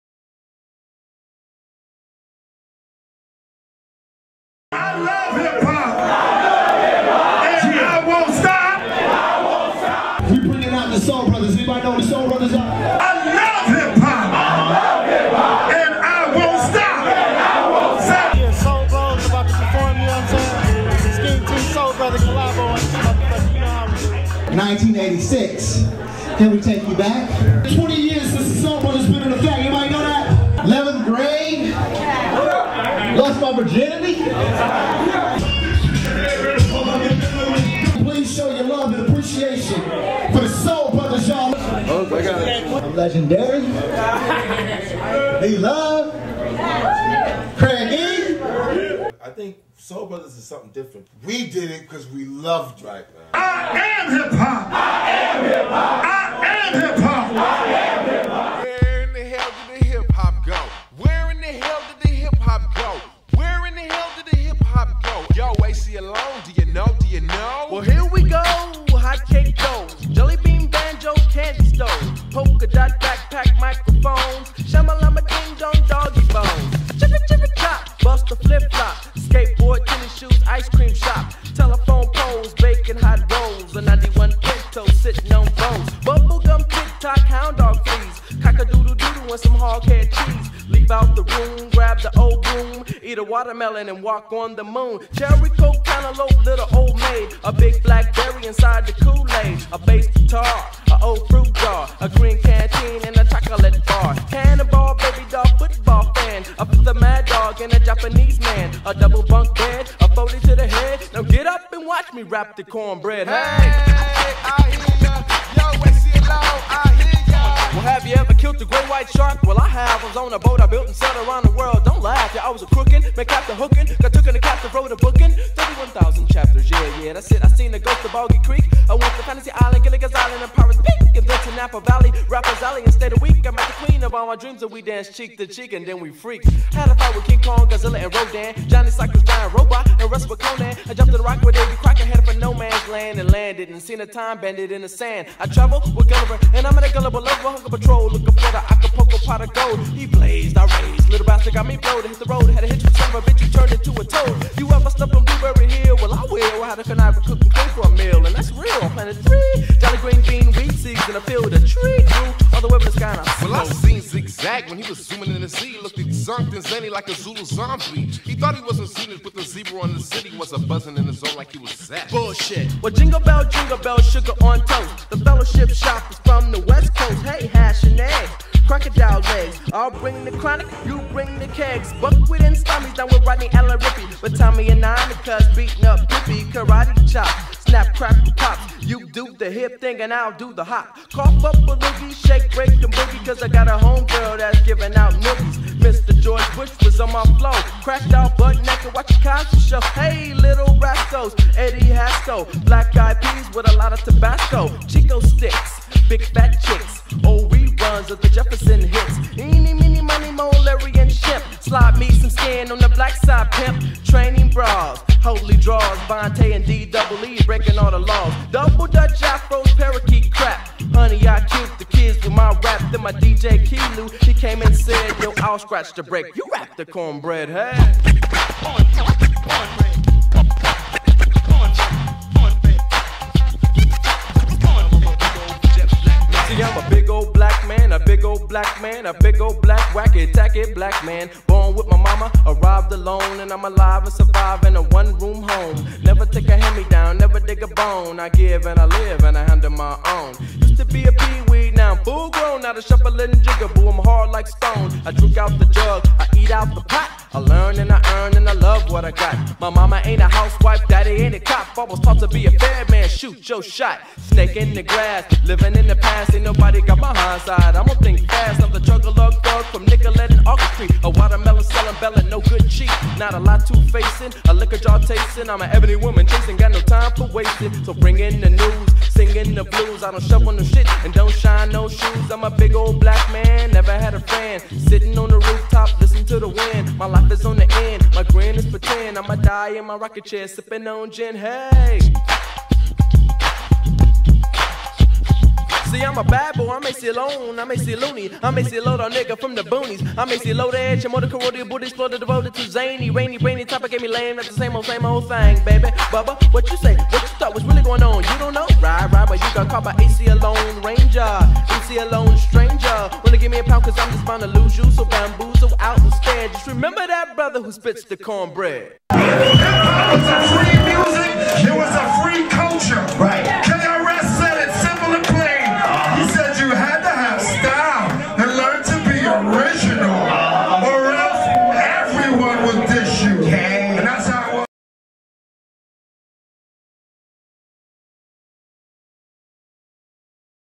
(REAL HIP HOP DANCE) The Golden Era By: Frankie.d &BrodieMacProductions
The Golden Era of HipHop..W/the Soul Brothers,Swat Team,Brother (SkeemTeam),PopCornChicken,A.J,Tish,Richmond/Tone,Fly Styles,Super Dave,Lunitic &many more..Its the evolution & transition of what's called Freestyle..1 reason I used Freestyle Fellowship (BigUp!!) for song..Real Hip Hop...The Trendy Dayz Movie coming soon!!